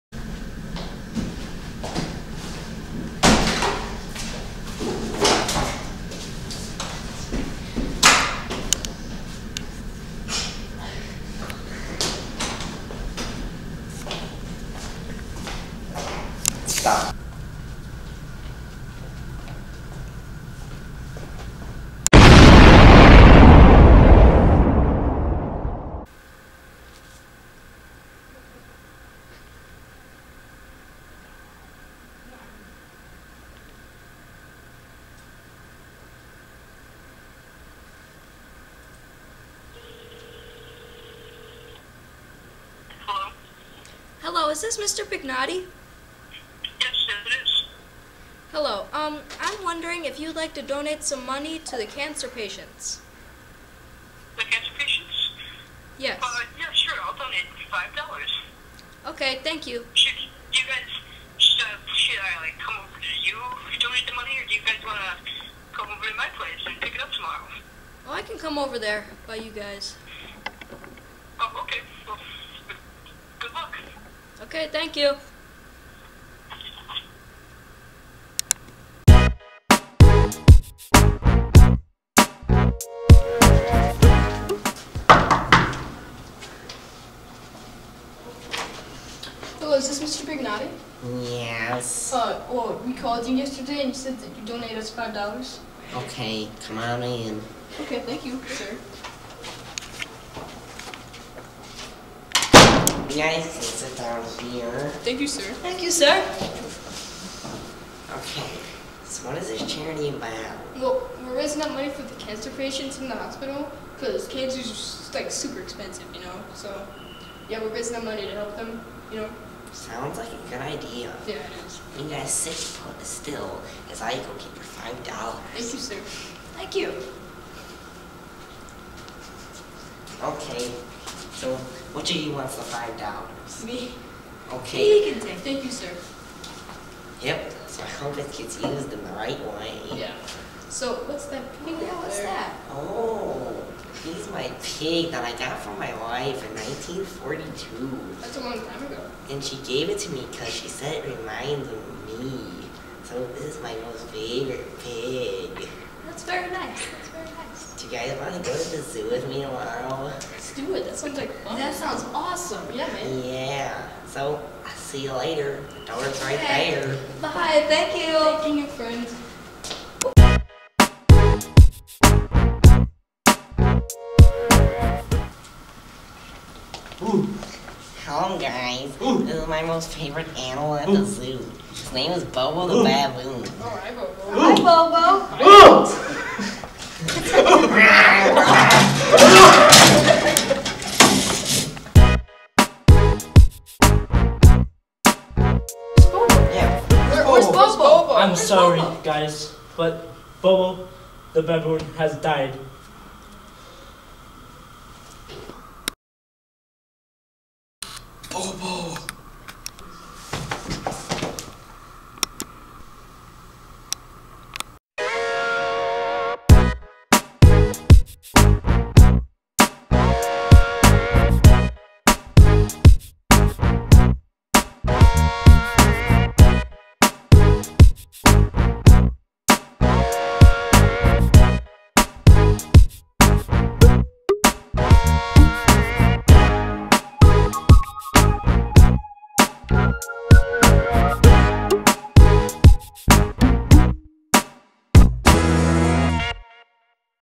THE PIGMAN BOOK TRAILER
This is a book trailer for the book: The Pigman by Paul Zindel